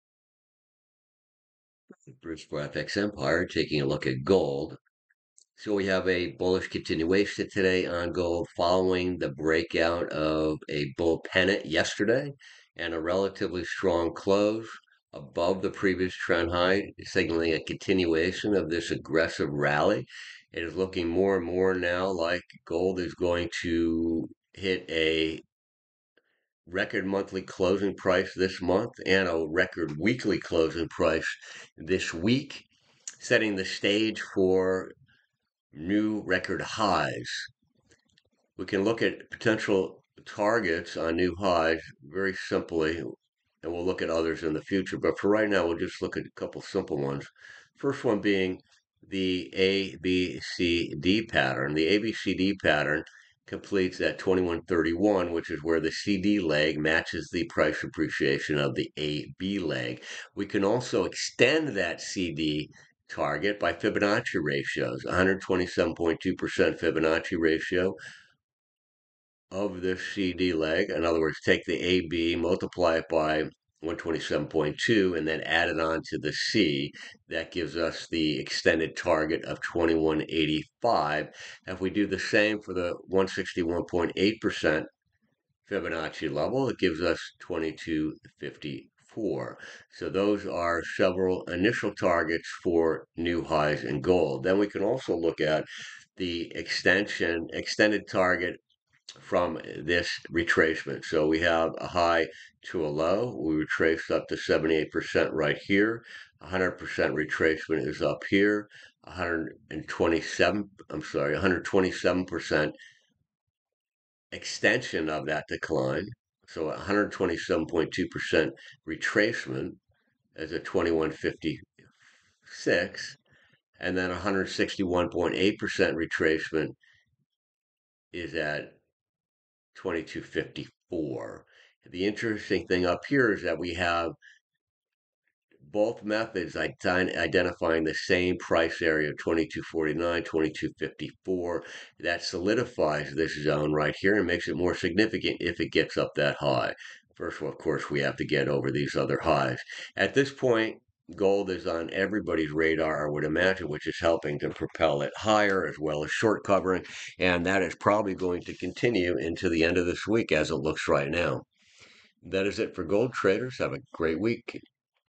Gold Forecast Video for 29.11.23 by Bruce Powers for FX Empire
Gold's surge, breaking resistance and eyeing record monthly and weekly closes, marks a remarkable rally, challenging previous highs and setting sights on new territory.